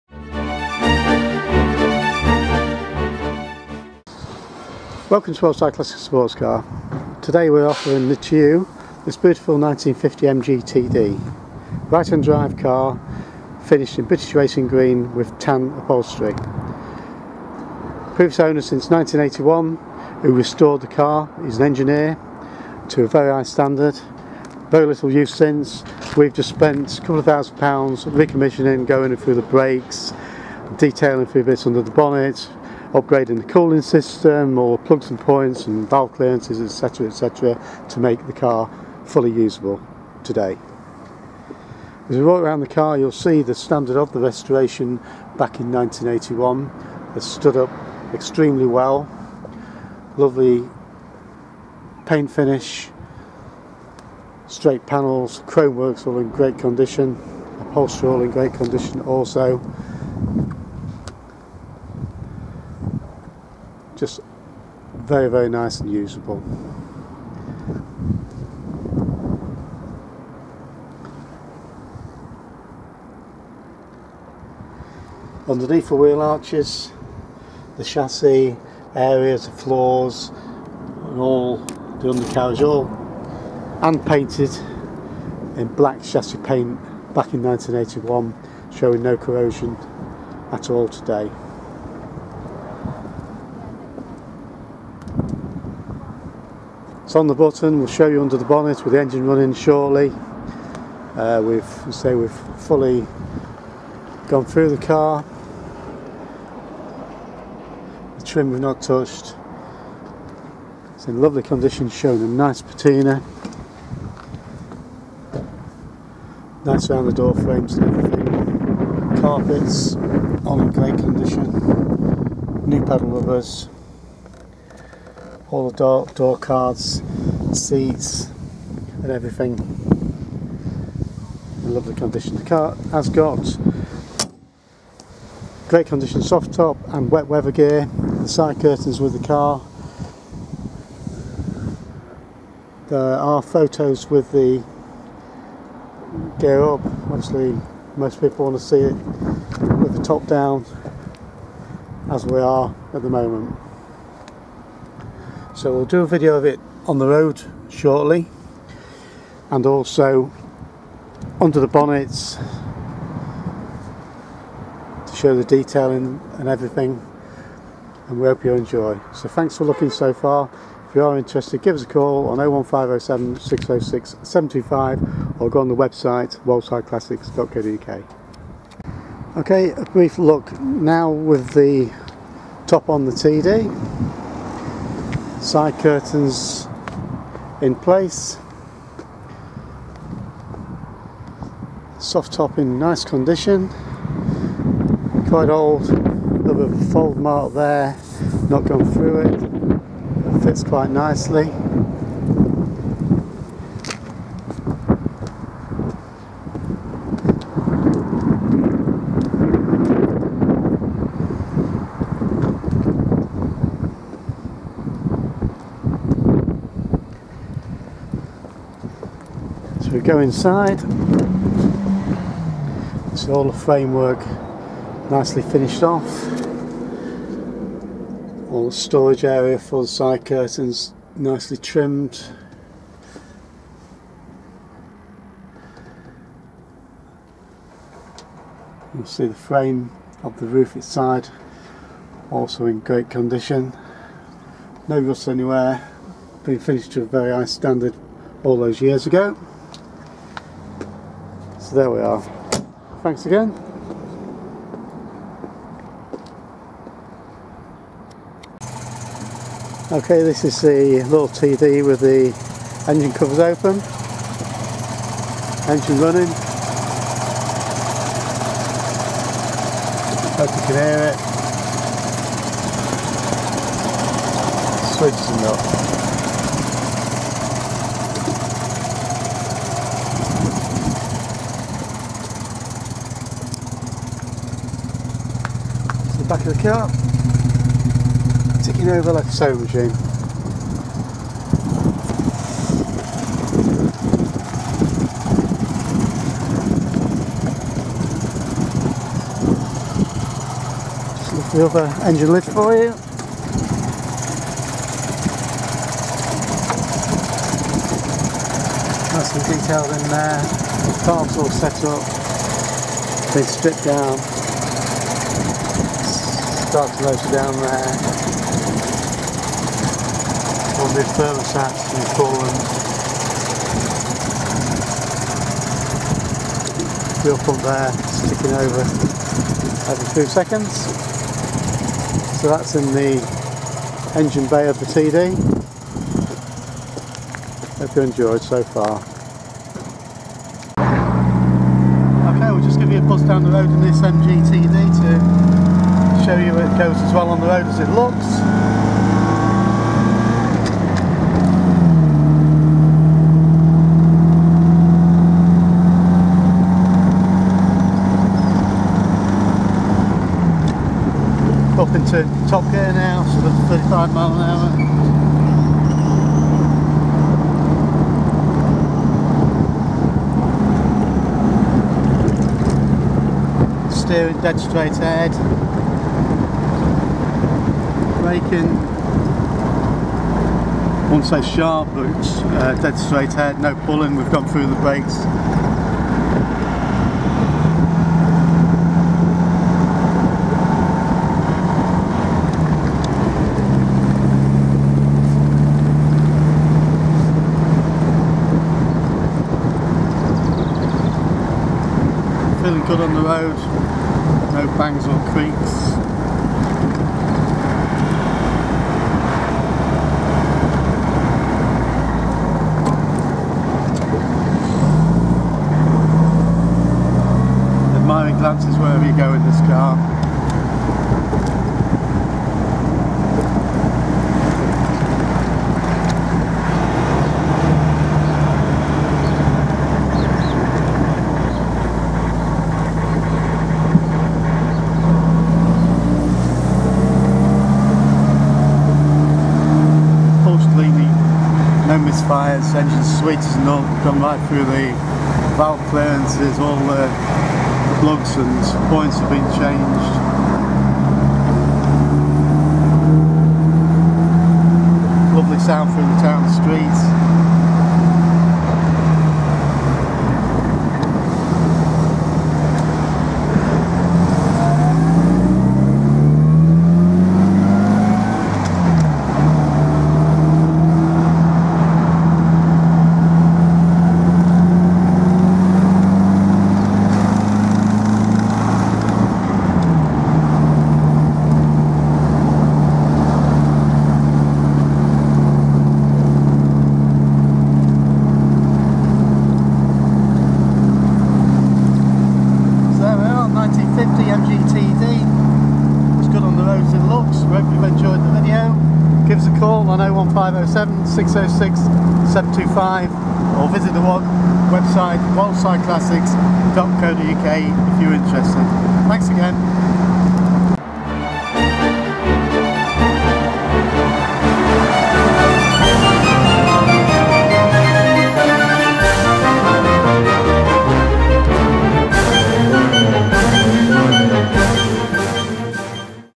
SOLD - 1950 MG TD Midget Roadster Classic Convertible for sale in Louth Lincolnshire
NOW SOLD -1950 MG TD Midget Roadster Classic Convertible in Bristisg Racing Green.  New in stock is this lovely RHD 1950 MGTD finished in British Racing Green with tan leather trim. The car was restored by the previous engineer owner in the early 1980's and still presents very well. With the car are the side curtains and wet weather gear.